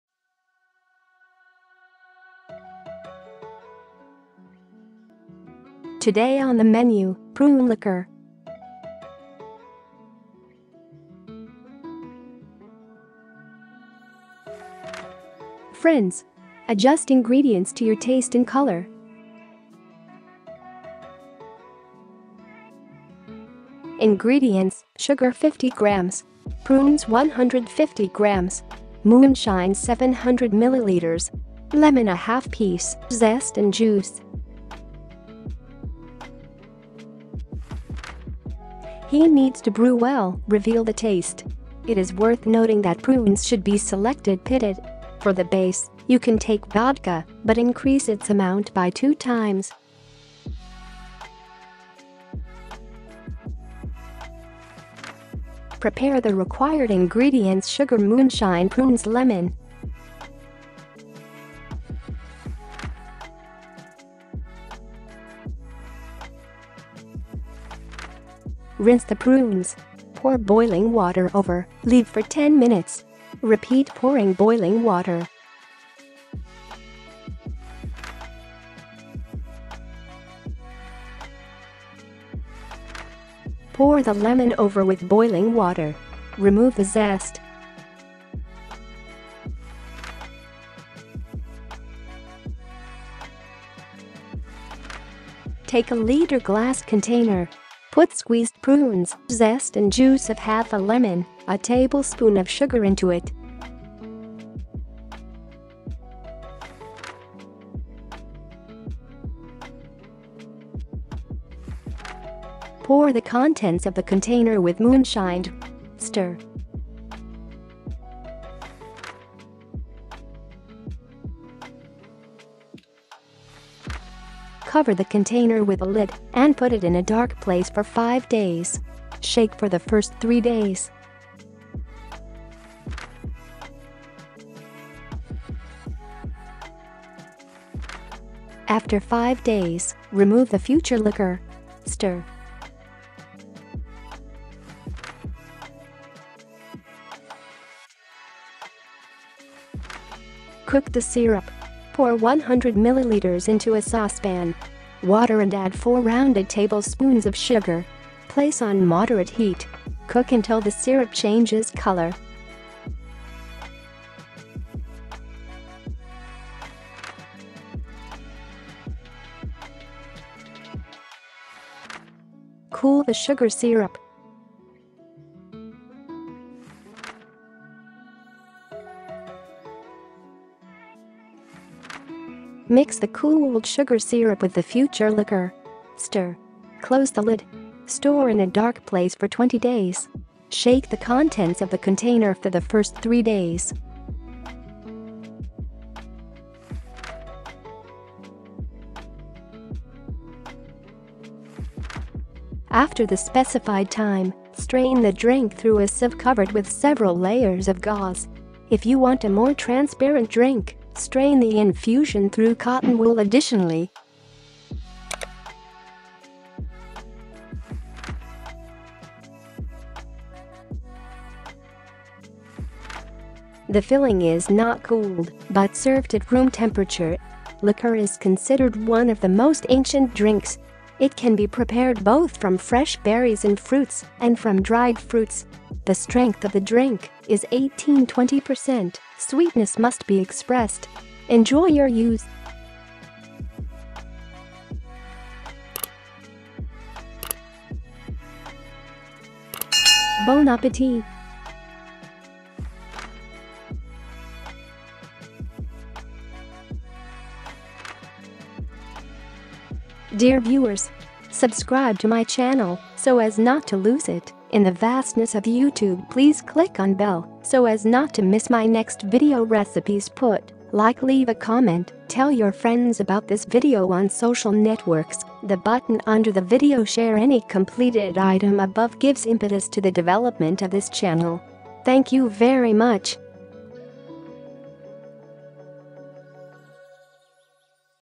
Prune Pouring / Book of recipes / Bon Appetit
✅ Prune Pouring 👇

Ingredients:
* Sugar - 50 Grams
* Prunes - 150 Grams
* Moonshine - 700 Milliliters
* Lemon - 1/2 Piece (zest and juice)

He needs to brew well, reveal the taste. It is worth noting that prunes should be selected pitted. For the base, you can take vodka, but increase its amount by 2 times.

👉 Step by step cooking 👇

1. Prepare the required ingredients sugar moonshine prunes lemon.

2. Rinse the prunes. Pour boiling water over, leave for 10 minutes. Repeat pouring boiling water.

3. Pour the lemon over with boiling water. Remove the zest.

4. Take a liter glass container. Put squeezed prunes, zest and juice of half a lemon, a tablespoon of sugar into it.

5. Pour the contents of the container with moonshine. Stir.

6. Cover the container with a lid and put it in a dark place for 5 days. Shake for the first three days.

7. After 5 days, remove the future liqueur. Stir.

8. Cook the syrup. Pour 100 ml into a saucepan. water and add 4 rounded tablespoons of sugar. Place on moderate heat. Cook until the syrup changes color.

9. Cool the sugar syrup.

10. Mix the cooled sugar syrup with the future liqueur. Stir. Close the lid. Store in a dark place for 20 days. Shake the contents of the container for the first three days.

11. After the specified time, strain the drink through a sieve covered with several layers of gauze. If you want a more transparent drink, strain the infusion through cotton wool additionally.

12. The filling is not cooled, but served at room temperature. Liqueur is considered one of the most ancient drinks. It can be prepared both from fresh berries and fruits, and from dried fruits. The strength of the drink is 18-20%, sweetness must be expressed. Enjoy your use!